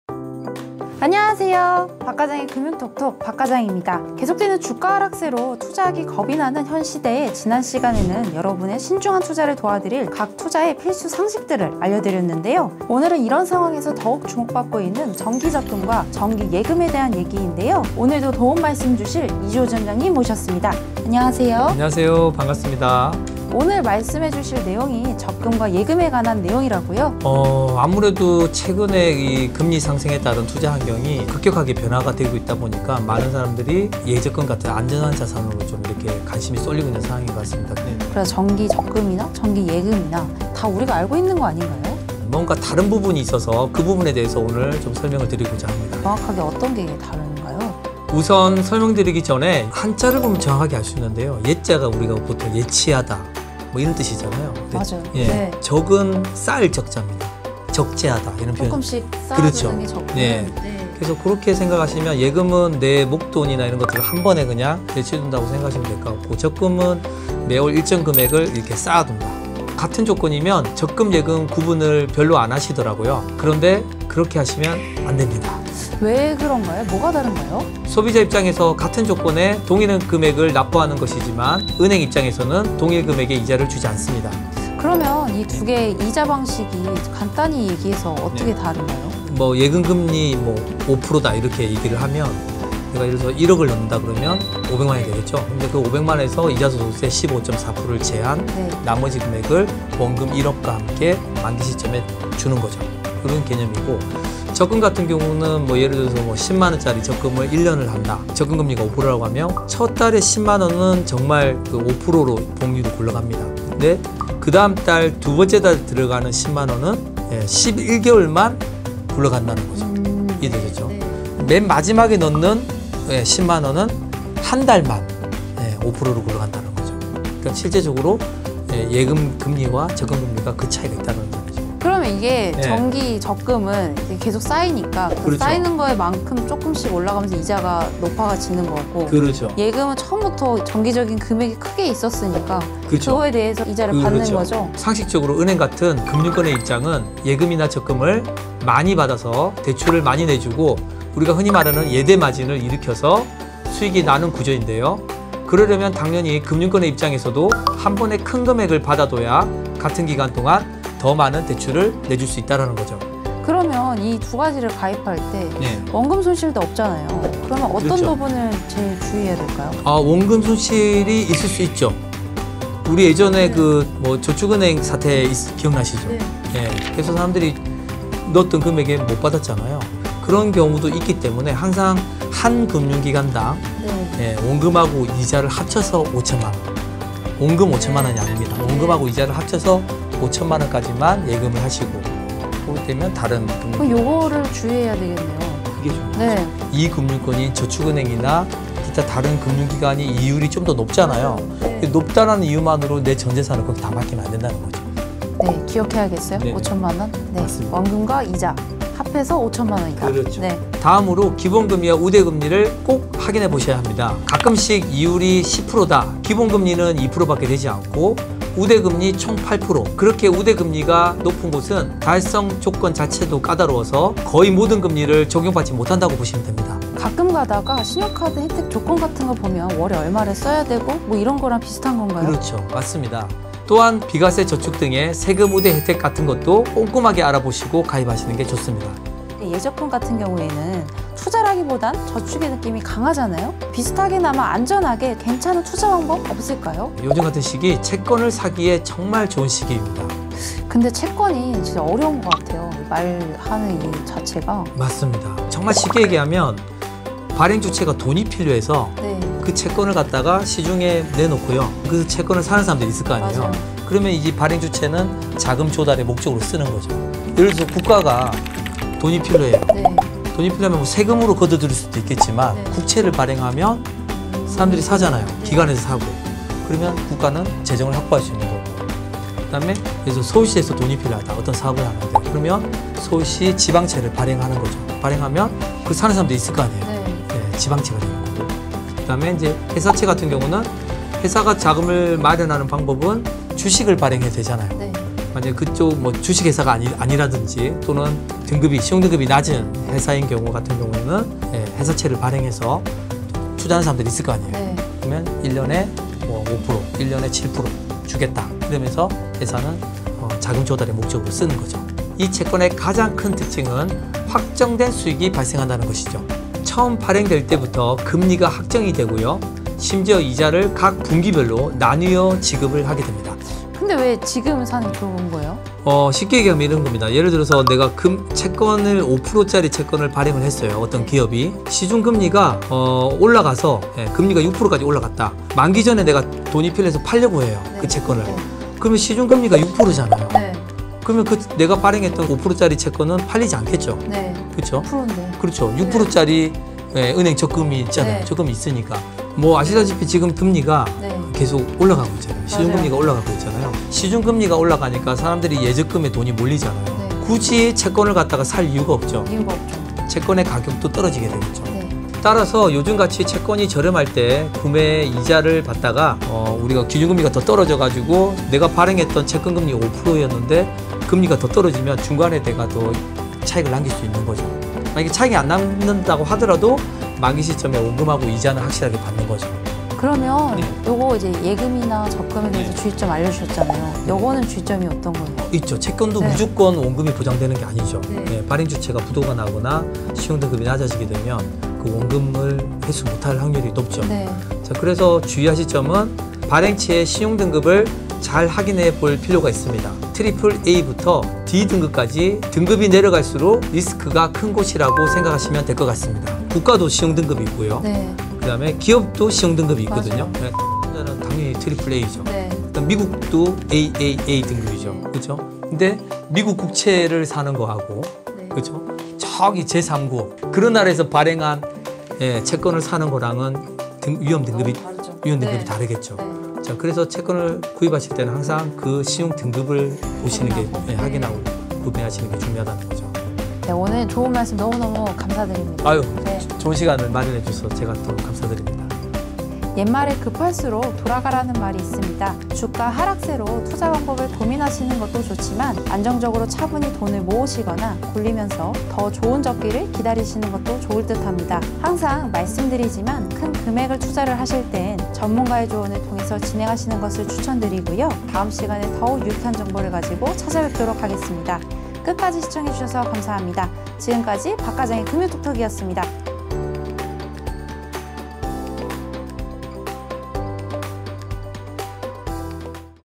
예적금도 원금손실이 있다고?!(ft 채권투자)
여러분, 일상생활에서 많이 들어봤던 예적금!
의미는 대충 알았어도, 이자 계산법에 어떤 차이가 있는지 혹시 알고 계셨나요?
그리고 예적금, 채권투자에도 원금손실의 리스크가 존재한다는 것도 알고 계셨나요?
오늘 영상 끝까지 보시고 여러분들 투자에 조금이나마 도움이 되었으면 좋겠습니다!

00:00 - 인트로
00:16 - 예금과 적금(차이점, 주의사항)
04:00 - 채권(의미, 주의사항)
07:15 - 아웃트로